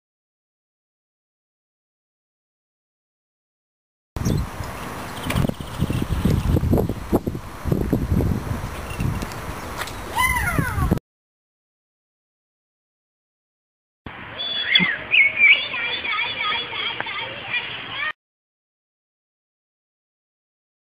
Puppy School Silvia Trkman Lesson 4 Part 1 ("the magic speed word") + Lesson 3 task 3 (cavaletti)
Border Collie Puppy Malaika (6 months) with Melinda at the railway station Ratzeburg and at the Lake Ratzeburg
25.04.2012, Tasks: "special version cavaletti" and "the magic turboword to increase speed"